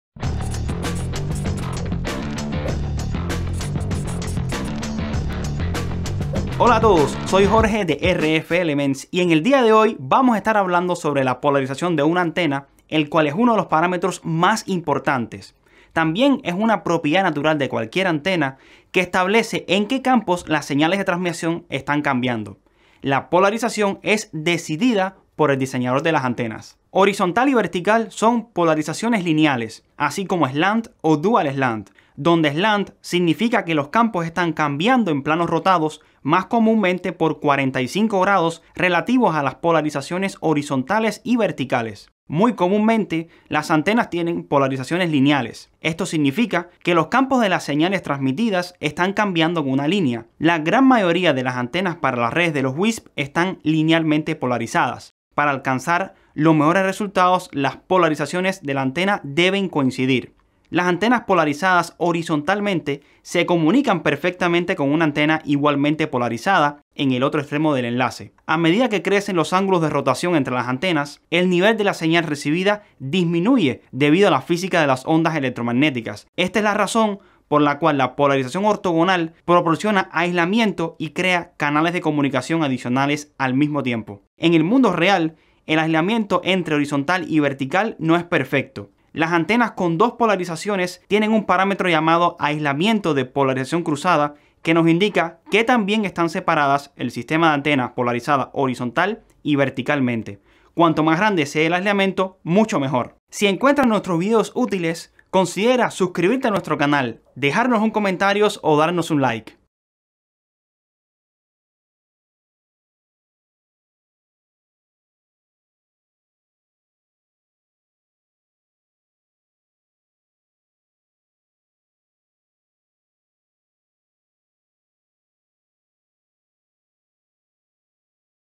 Inside Wireless en Español: Polarización de la antena
En este episodio, aprenderá sobre la polarización de la antena: qué es, cómo funciona, por qué es importante y qué polarización es típica de las antenas WISP.

La polarización establece en que campos las señales de transmisión están cambiando. La polarización es decidida por el diseñador de las antenas. Horizontal y vertical son polarizaciones lineales, así como Slant o Dual Slant.

La gran mayoría de las antenas para las redes de los WISPs están linealmente polarizadas. Para alcanzar los mejores resultados las polarizaciones de la antena deben coincidir.  Las antenas polarizadas horizontalmente se comunican perfectamente con una antena igualmente polarizada en el otro extremo del enlace.

Las antenas con dos polarizaciones tienen un parámetro llamado aislamiento de polarización cruzada que nos indica qué tan bien están separados el sistema de antena polarizada horizontal y verticalmente.

Visite para obtener más información sobre la tecnología de antenas de RF elements.

0:00 Introducción
00:19 Concepto
00:27 Tipos de polarización
1:24 Aislamiento de polarización cruzada
1:43 Fin